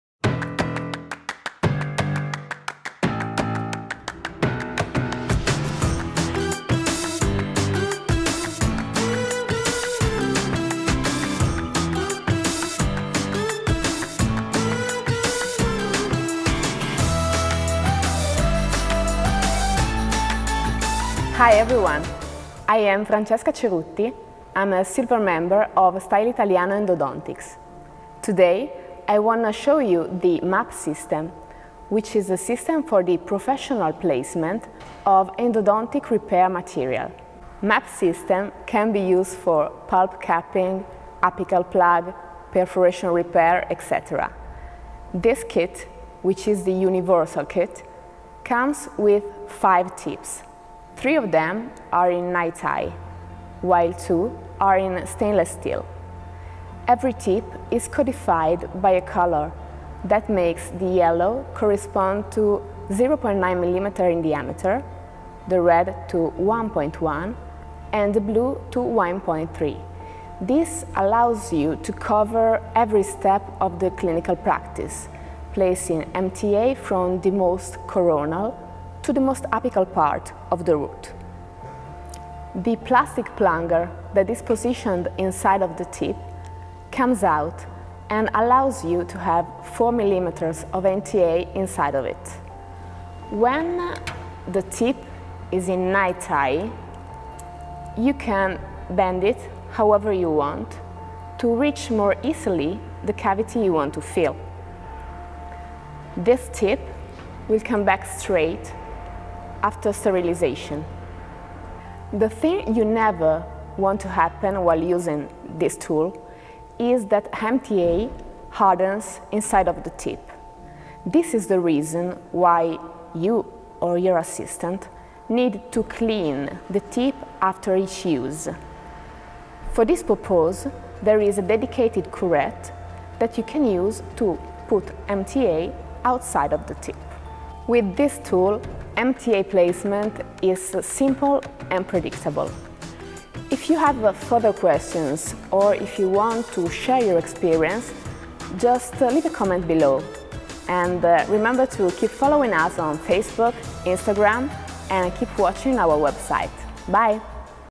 Unboxing MAP System
Francesca Cerutti, Style Italiano Endodontics Silver Member, presents the MAP System, by PD

Follow our Channel!